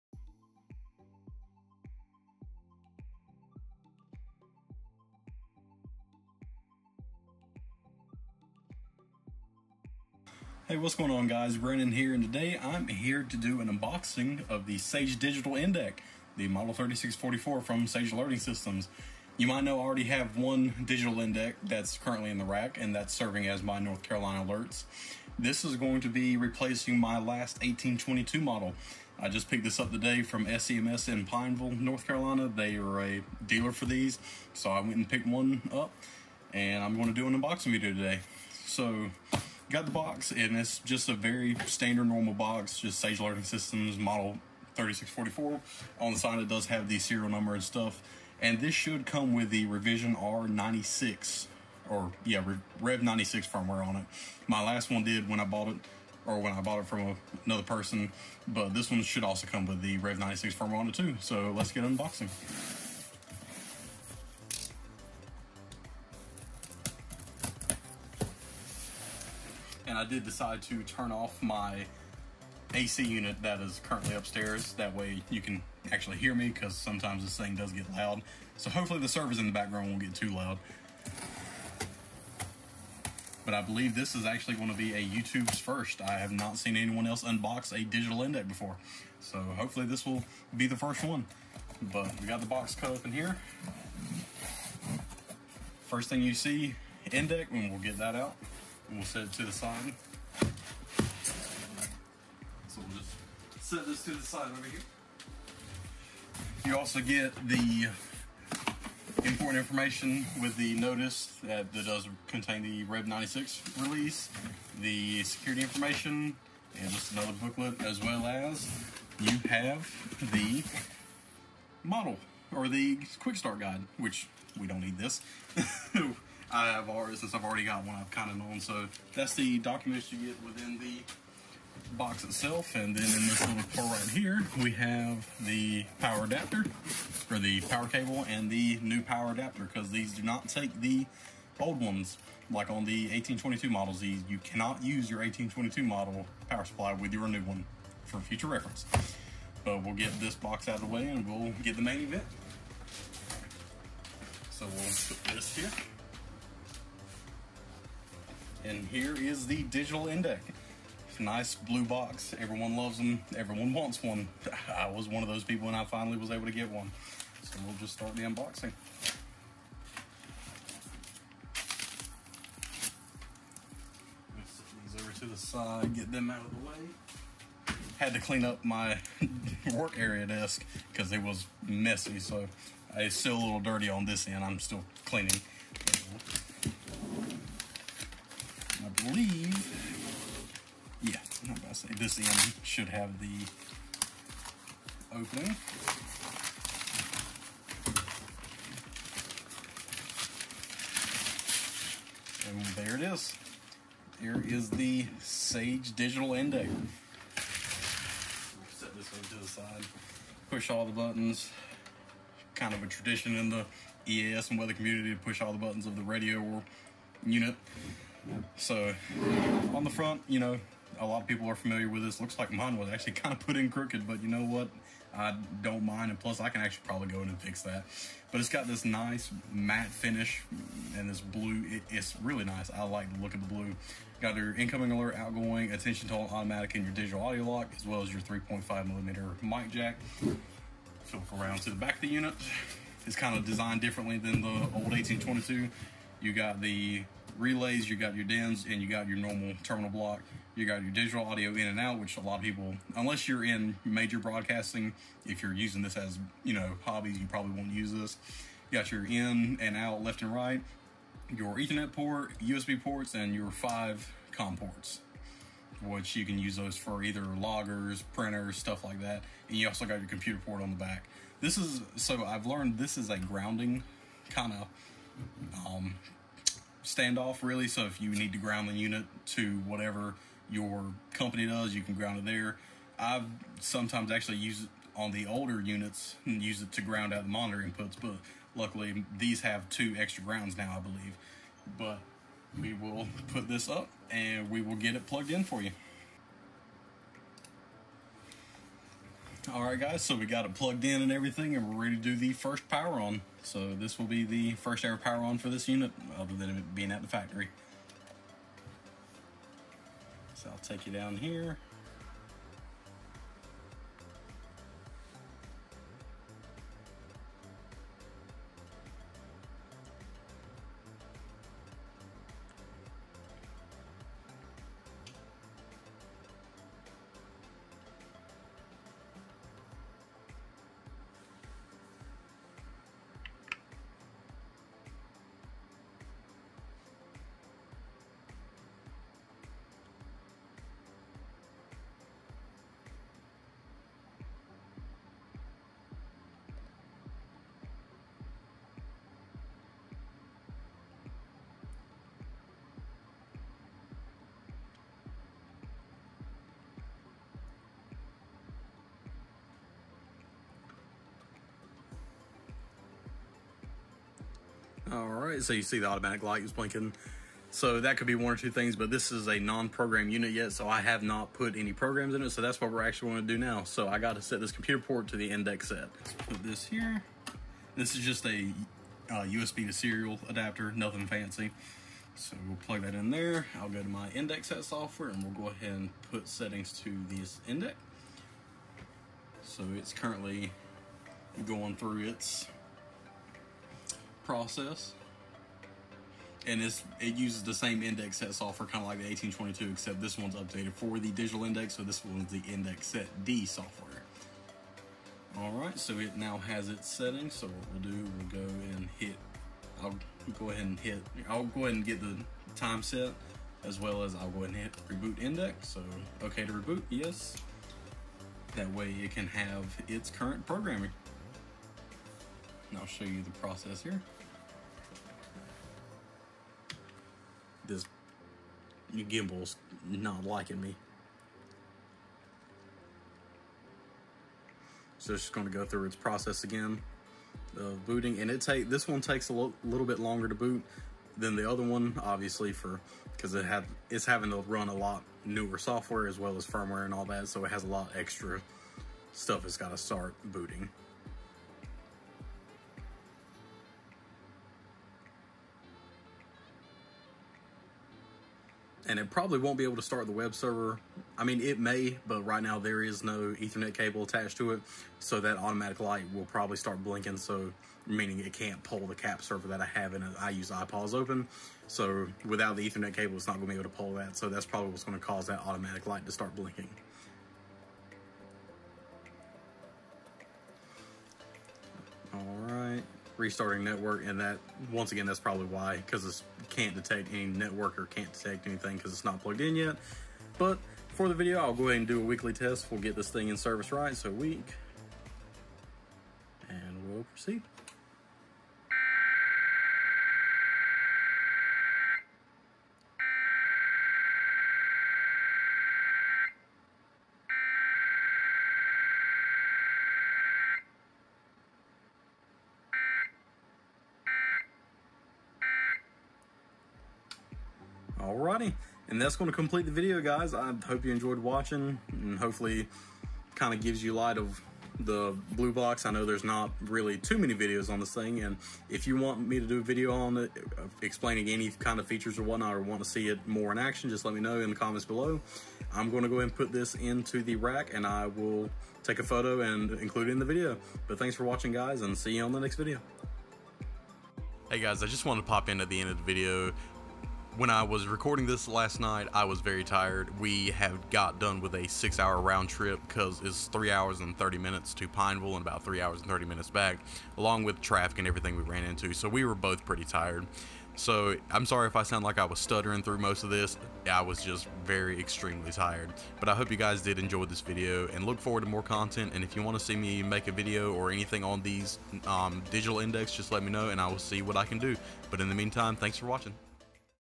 [YOUTUBE FIRST] SAGE Digital ENDEC 3644 Unboxing!
Hey Guys! Glad it could be a YouTube's First! Once again I apologize for my stuttering, it was a long drive to get this unit and I was tired out after!

Video Timestamps:
00:00 - Introduction
01:02 - Opening the Box
01:33 - Taking Everything Out
02:37 - The Main Event, The Blue Box!
03:39 - Tour Around the Unit
04:56 - First Power On
06:15 - Programming Unit via EndecSetD
08:11 - Sending the first RWT
08:37 - Outro/More Information